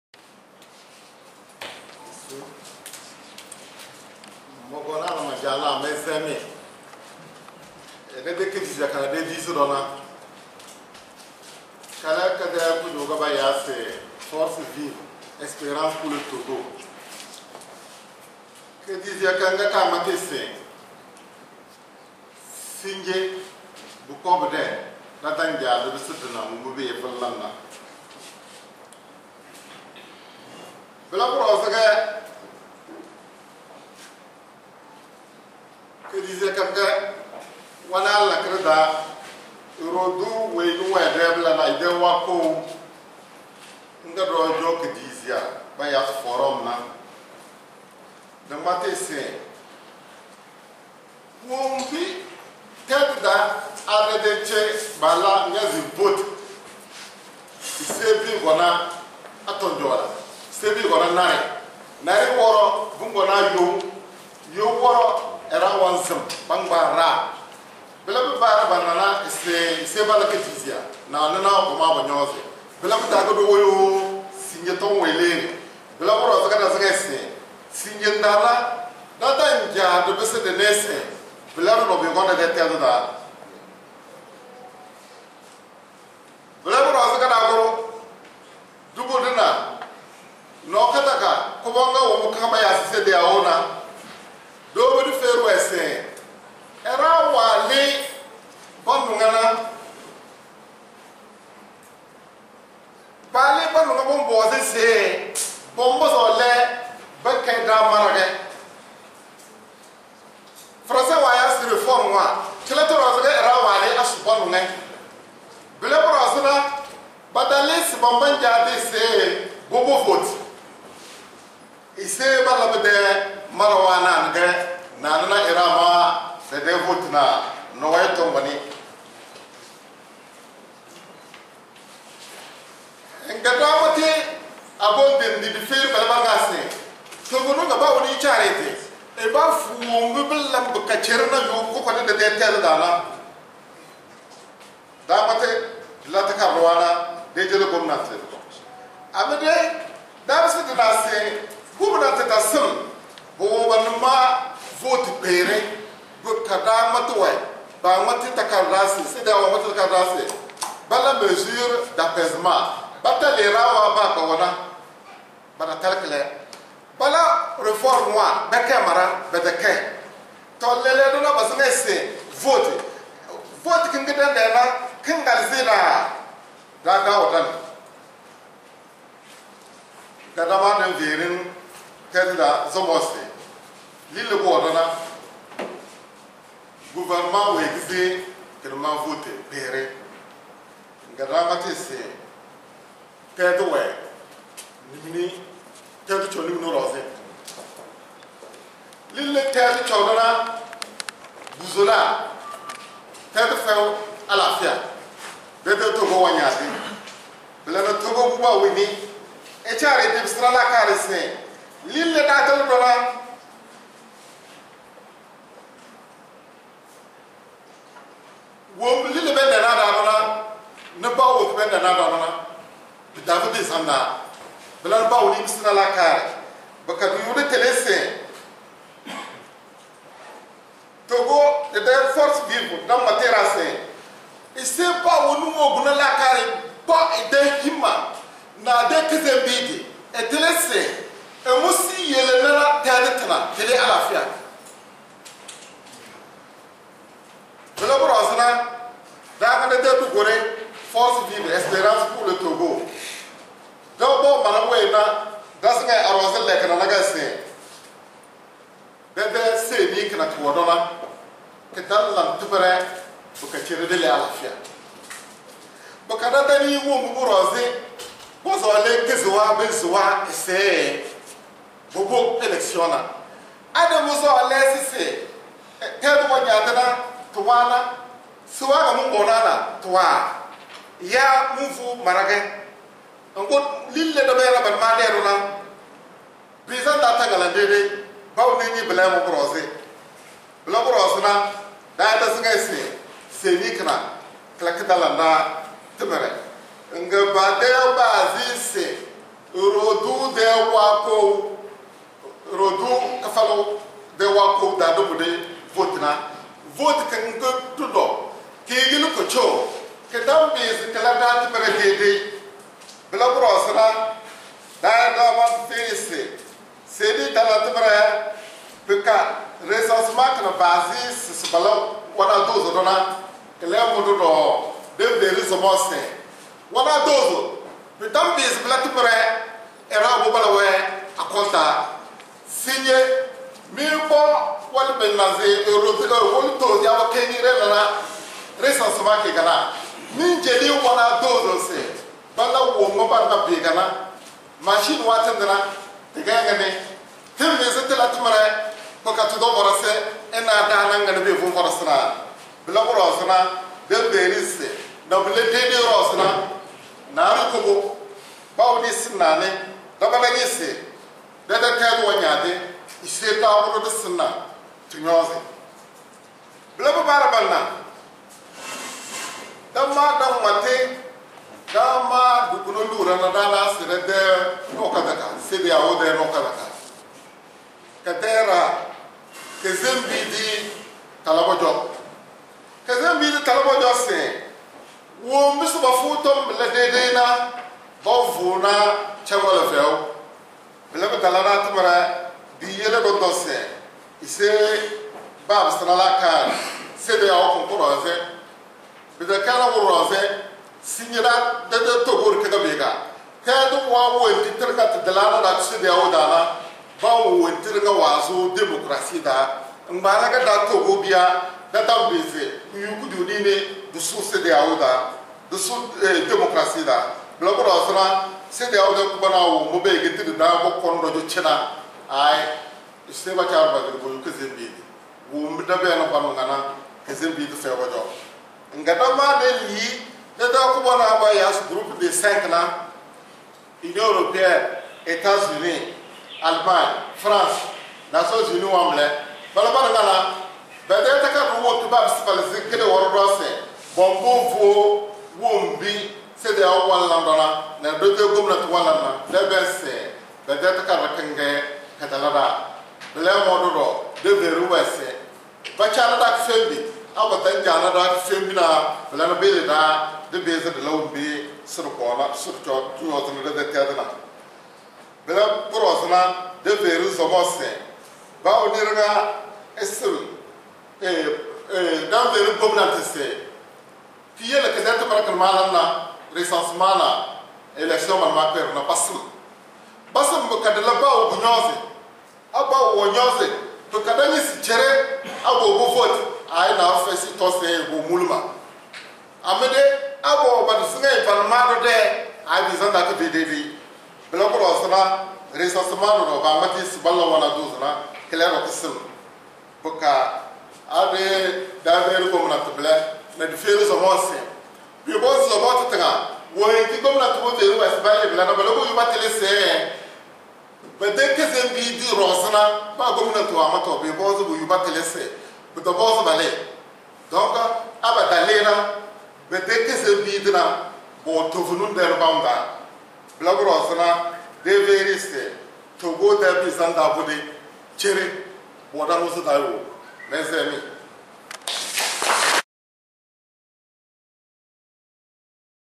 TOGO/Les Forces Vives "Espérance pour le Togo" demande l'arrêt du processus électoral
15 Novembre 2018: Le Mouvement des Forces Vives "Espérance pour le Togo"  regroupant certaines organisations de la Société civile togolais a protesté vivement contre la dernière sortie des diplomates du Groupe des cinq à savoir les USA, l'UE, l'Allemagne, la France et le Système des Nations Unies au Togo qui dans une déclaration conjointe en début de semaine appelle les populations à aller se faire enregistrer au cours des trois jours de prolongation du recensement qu'à lancer la Commission électorale Nationale Indépendante, CENI toujours incomplète.
Pendant ce temps, la C14 et plusieurs organisations de la Société civile appelle à l'arrêt du processus électoral et au respect de la feuille de route de la CEDEAO.